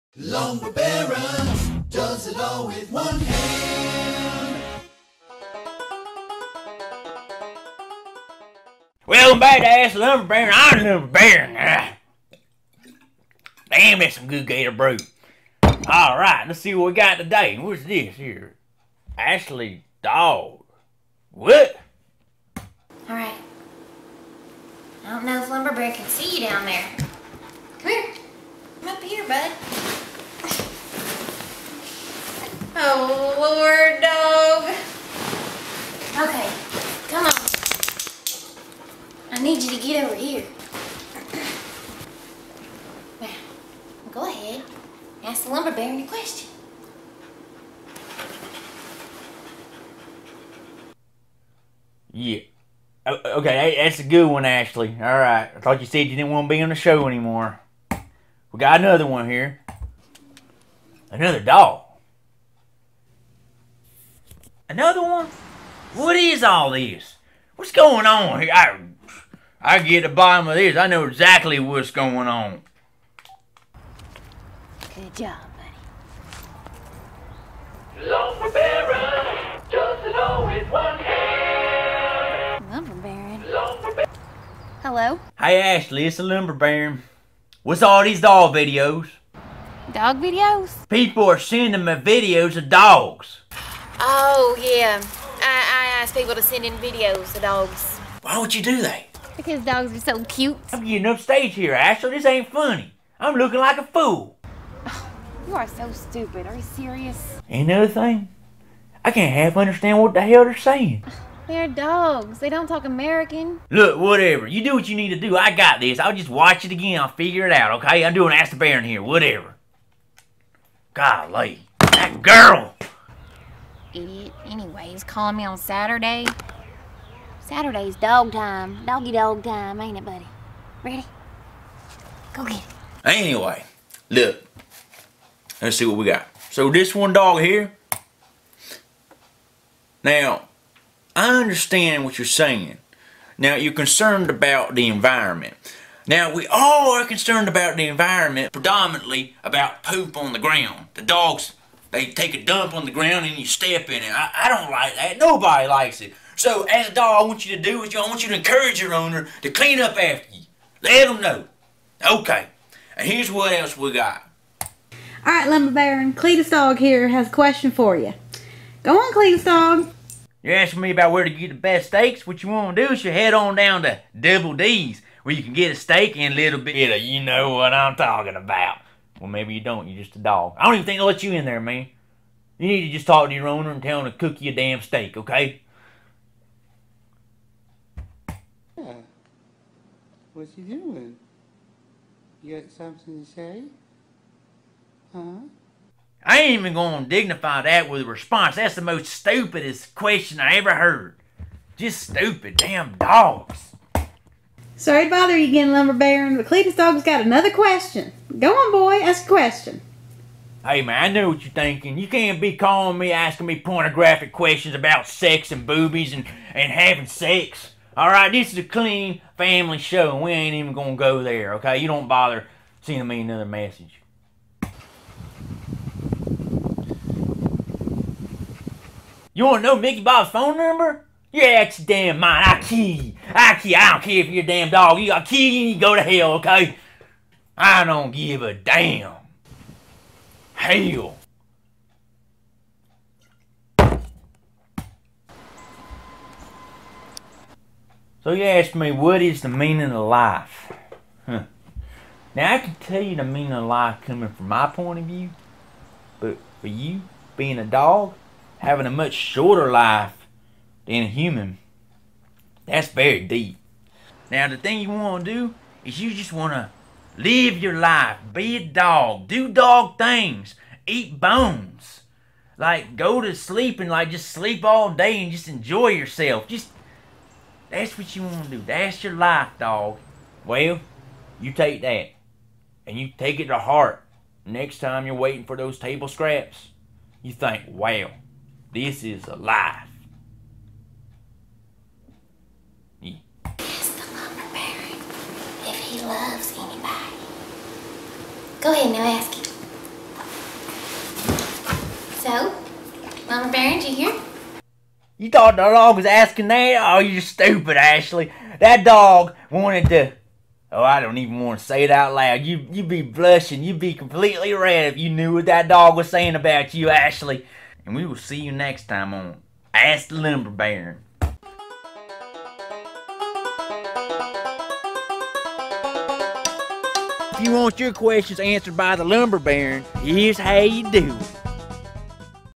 Ask the Baron 16: "Dogs of Jasper County"
Ashley asks dog owners to send in clips of their pets. The Lumber Baron attempts to answer the dogs' "questions."